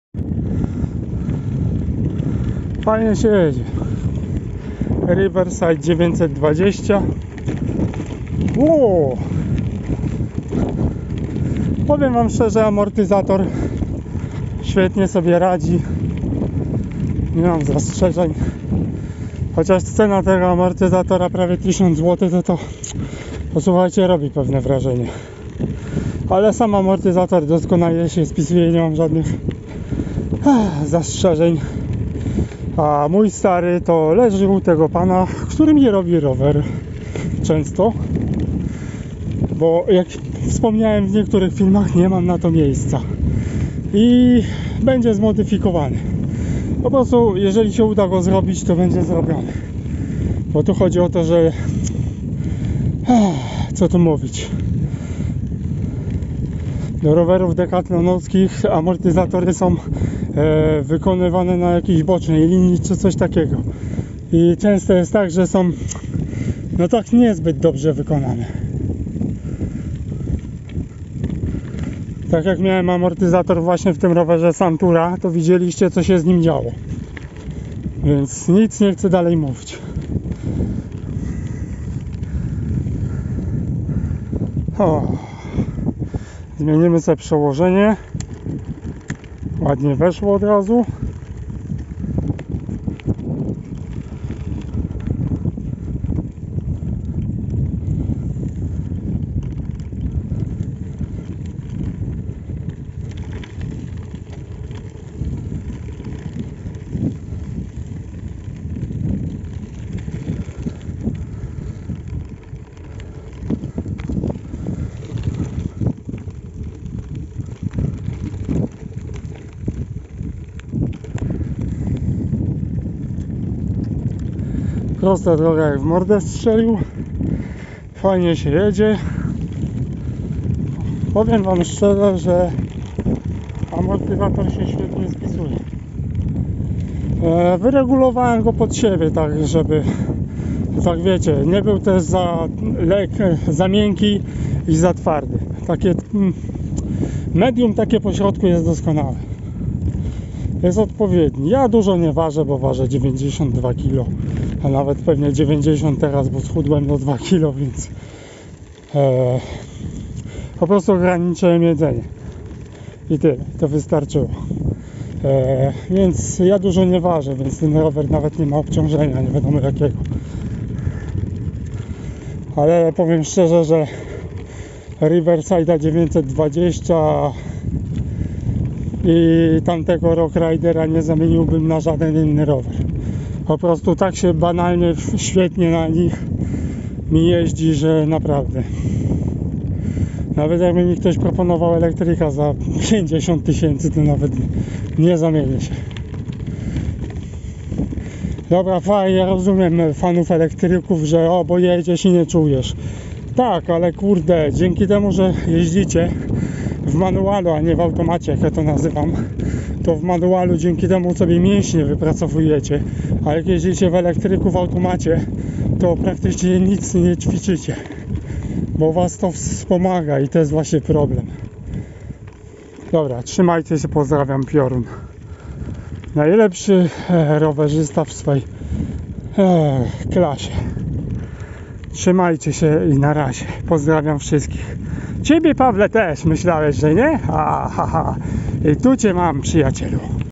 Jazda Riverside 920
RIVERSIDE
Model:920
Sugerowana cena *3199.00 PLN

TECHNOLOGIE
stery 1-1/8 cala (28,575 mm) stery bezgwintowe (a-head) stery półzintegrowane
WAGA
12.90 kg
RAMA
DOSTĘPNE ROZMIARY S M L
OPIS Aluminium 6061
MATERIAŁ Aluminium
WIDELEC Spinner 300S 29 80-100 mm (cross country, maratony, turystyka)
SKOK WIDELCA 100 mm
NAPĘD
GRUPA OSPRZĘTU SRAM NX
PRZERZUTKA TYLNA SRAM NX
ILOŚĆ BLATÓW I KORONEK '1x11
MECHANIZM KORBOWY 36 zębów SRAM NX
KASETA SRAM NX
ŁAŃCUCH KMC X11
PEDAŁY plastik
KOŁA
ROZMIAR KÓŁ 28" (700C)
OBRĘCZE Mavic CROSSRIDE
OPONY Trekking speed 700 x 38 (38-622)
KONTROLA
MANETKI SRAM NX
HAMULCE Btwin by PROMAX DSK 908
TYP HAMULCÓW Tarczowe hydr.
KIEROWNICA 620 mm) z aluminium
MOSTEK 31,8 mm) z aluminium
STERY ahead set 1"1/8, pół-zintegrowane
SIODEŁKO B'TWIN ERGOFIT SPORT 900
WSPORNIK SIODEŁKA aluminium